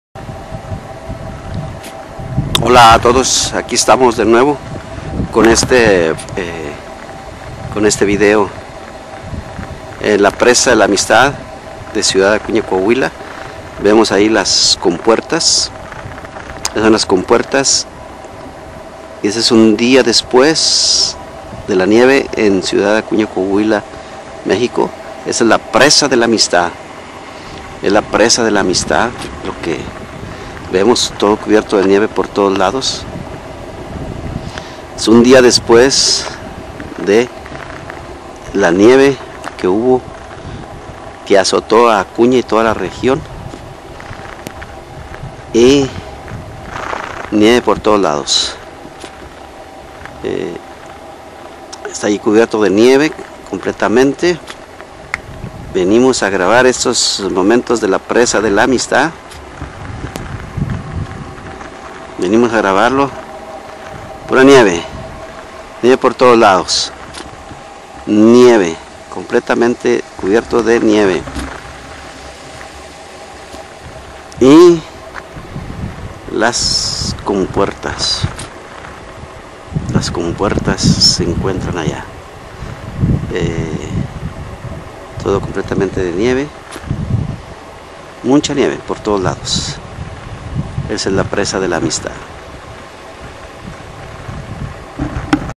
La Presa de la Amistad cubierta de nieve y hielo
este 19 de febrero del 2021 un dia despues de la nieve como amanecio la Presa de la Amistad